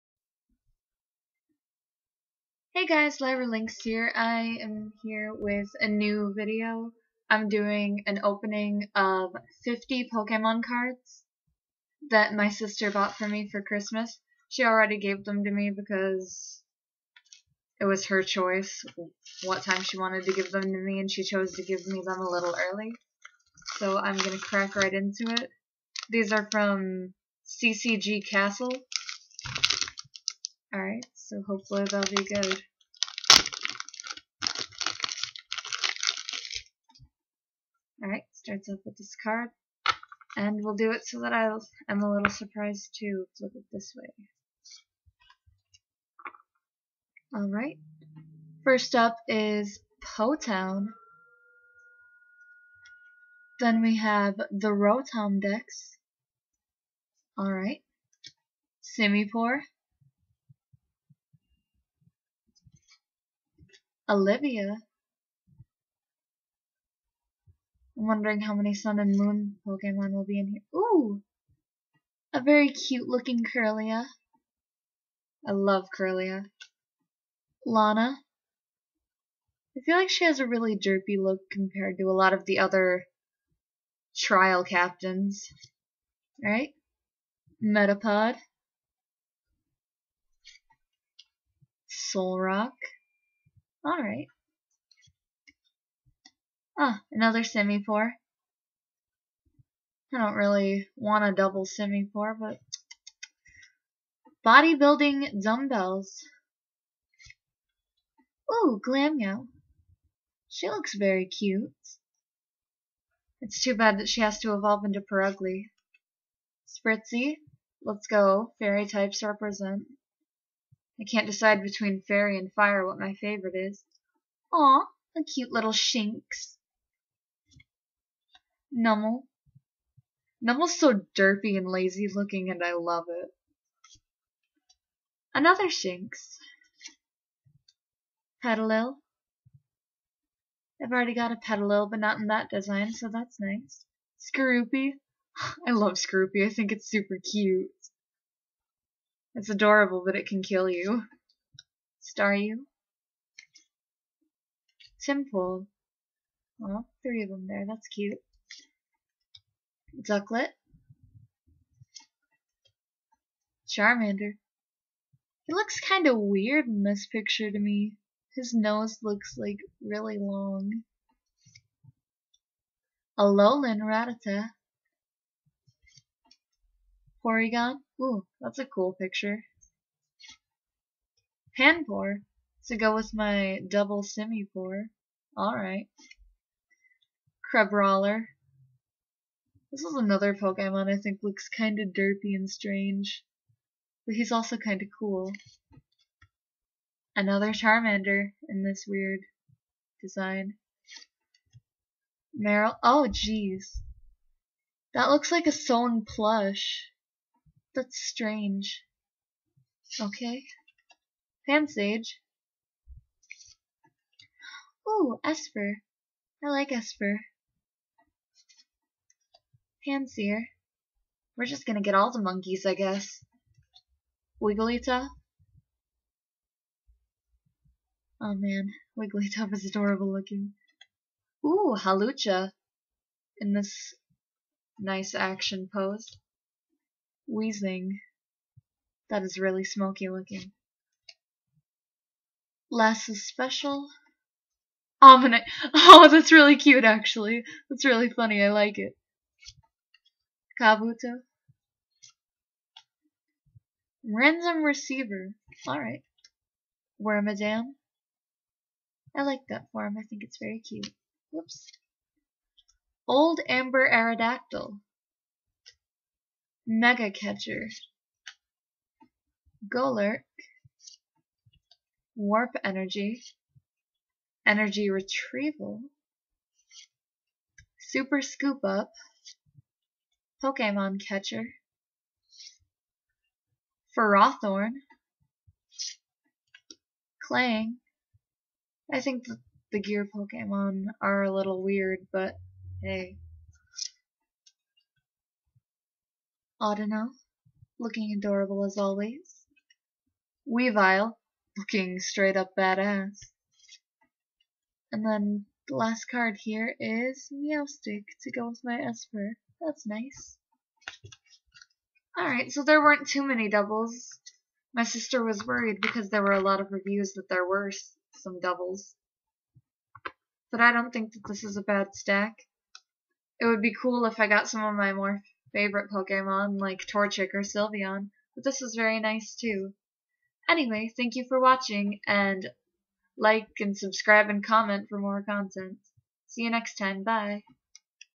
50 Card Pokemon Pack Opening
A Christmas present from BloodyGrey. Sorry. I don't have the best quality equipment yet, but I was excited about the gift and wanted to share in the fun.